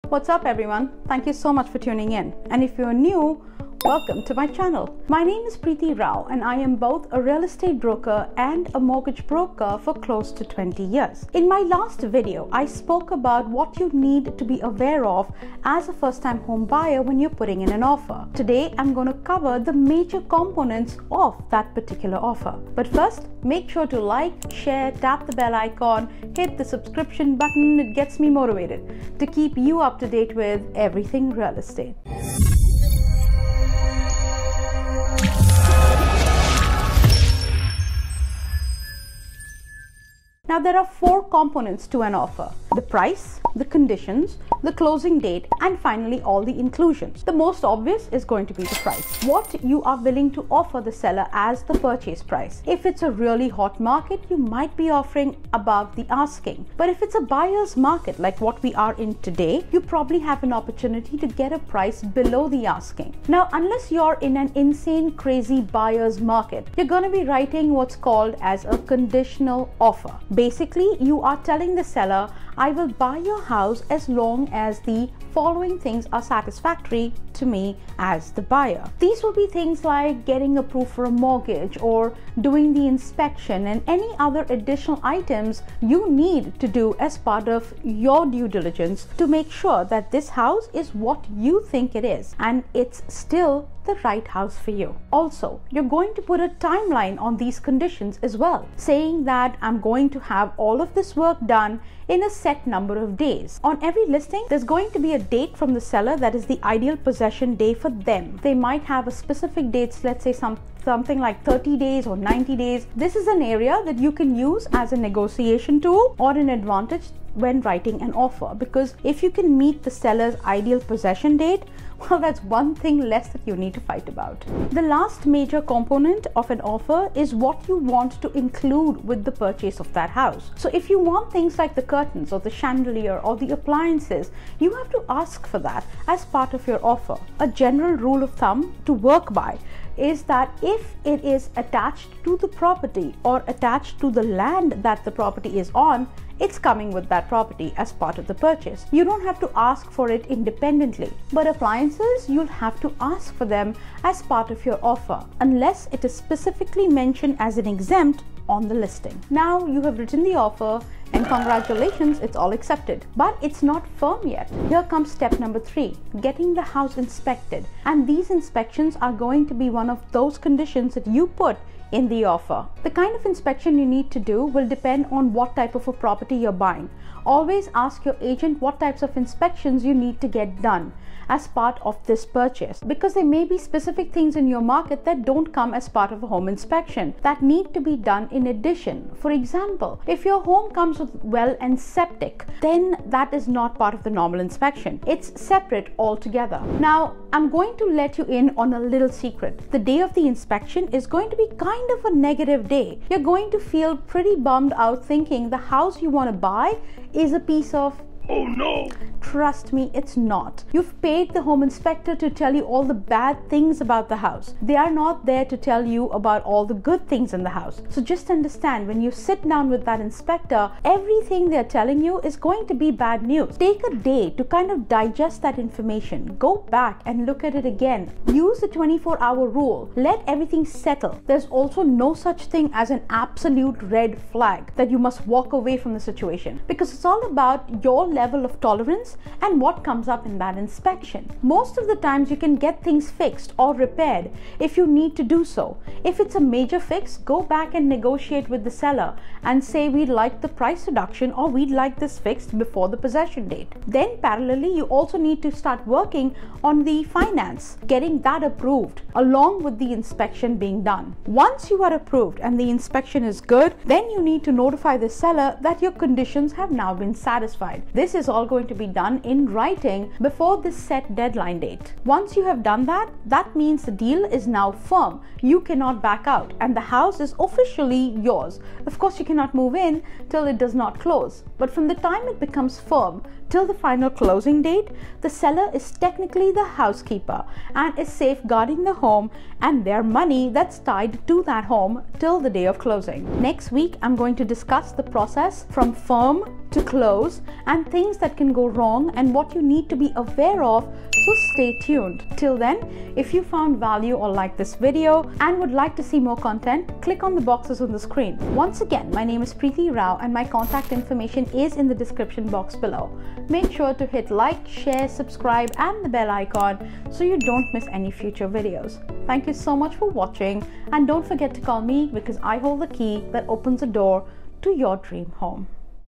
Complete Home Buying Process - Buying A House For The First Time - PART 2
The home-buying process is the process of finding, negotiating, and purchasing a home. It involves researching the market, finding a real estate agent, getting pre-approved for a mortgage, finding a home, making an offer, finalizing the purchase, and closing the deal.

In my previous video, I discussed what you need to be aware of, as a first-time home buyer when you are putting in an offer.  In this video, I have discussed the major components of the offer on a home. Watch the video till the end to have a better understanding of the things involved in the home-buying process and how the whole process works in a flow. I hope you find this video helpful and love it!

Chapters:
00:00 - Introduction
00:40 - Components of an offer
01:07 - Insane crazy buyers' market
02:45 - Getting the house Inspected
03:48 - 24-hour rule
04:16 - Working on the Finance
04:53 - Conclusion